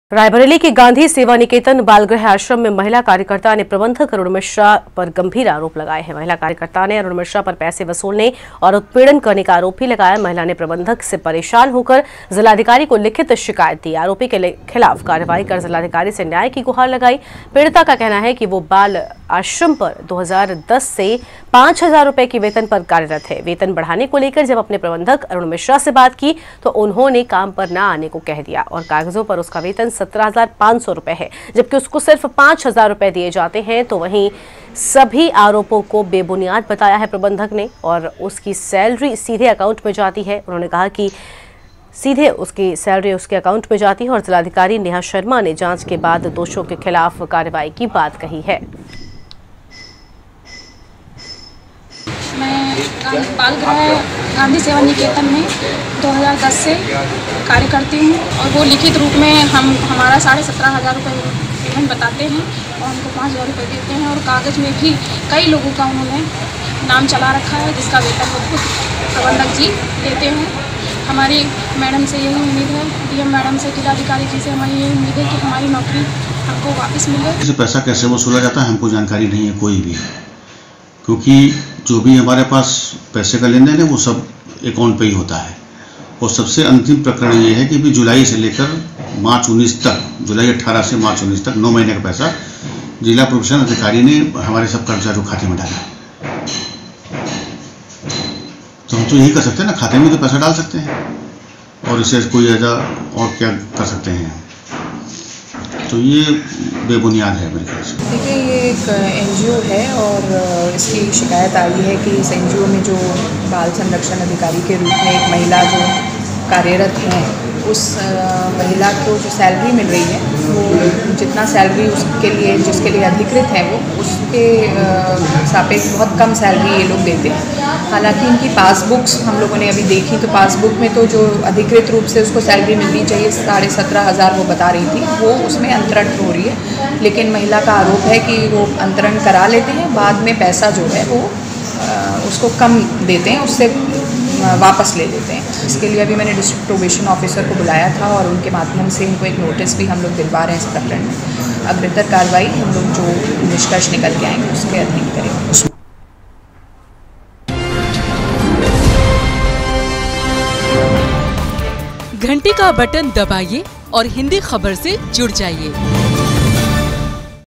बालगृह आश्रम में प्रबंधक पर लगे गंभीर आरोप, पीड़िता ने डीएम से की लिखित शिकायत
रायबरेली के गांधी सेवा निकेतन बालगृह आश्रम में महिला कार्यकर्ता ने प्रबंधक
अरुण मिश्रा गंभीर आरोप लगाये। महिला कार्यकर्ता ने अरुण मिश्रा पर पैसे वसूलने और उत्पीड़न करने का आरोप भी लगाया। महिला ने प्रबंधक से परेशान होकर जिलाधिकारी को लिखित शिकायत दी।  आरोपी के खिलाफ कार्रवाई कर जिलाधिकारी से न्याय की गुहार लगाई।